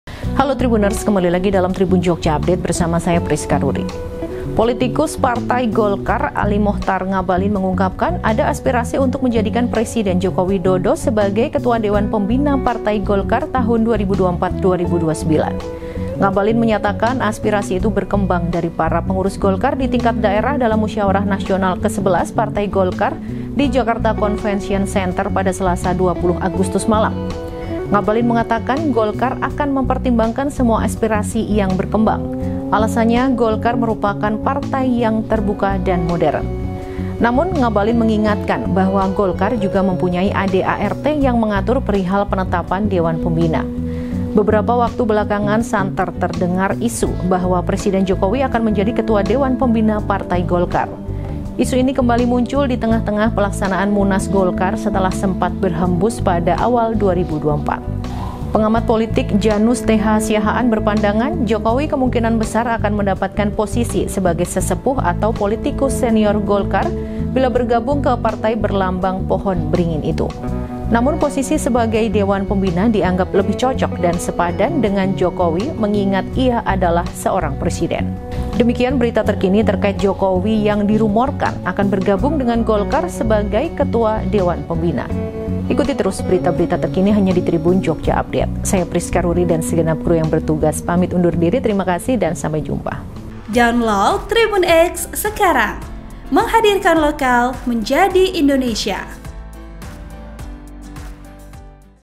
ASPIRASI JADIKAN JOKO WIDODO SEBAGAI KETUA DEWAN PEMBINA GOLKAR
Politikus Partai Golkar Ali Mochtar Ngabalin mengungkapkan, ada aspirasi untuk menjadikan Presiden Joko Widodo sebagai Ketua Dewan Pembina Partai Golkar tahun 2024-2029.

Ngabalin menyatakan, aspirasi itu berkembang dari para pengurus Golkar di tingkat daerah dalam Musyawarah Nasional XI Partai Golkar, Jakarta Convention Center, Selasa (20/8/2024) malam.

Setuju belum setuju, urusan belakang itu, kata Ngabalin melanjutkan.

Ngabalin mengatakan, Golkar akan mempertimbangkan semua aspirasi yang berkembang.

Alasannya, Golkar merupakan partai yang terbuka dan modern.

Namun, Ngabalin mengingatkan bahwa Golkar juga mempunyai AD/ART yang mengatur perihal penetapan Dewan Pembina.

Beberapa waktu belakangan santer terdengar isu bahwa Presiden Jokowi akan menjadi Ketua Dewan Pembina Partai Golkar.

Isu ini kembali muncul di tengah-tengah pelaksanaan Munas Golkar setelah sempat berembus pada awal 2024.

Pengamat politik Jannus TH Siahaan berpandangan, Jokowi kemungkinan besar akan mendapatkan posisi sebagai sesepuh atau politikus senior Golkar bila bergabung ke partai berlambang pohon beringin itu.

Boleh jadi bukan untuk menjadi Ketua Umum DPP Partai Golkar, tapi sebagai Dewan Pembina dan sejenisnya, yang membuat posisi Jokowi secara simbolis lebih senior ketimbang seorang ketua umum, kata Jannus, 5 Maret 2024.

Posisi sebagai Dewan Pembina dianggap lebih cocok dan sepadan dengan Jokowi mengingat dia adalah presiden.

Editor video : Afifudin
Editor Naskah : Bramasto Adhy
Narator : Prisca Ruri
Produser : Ribut Raharjo

NB

Sumber: Kompas.com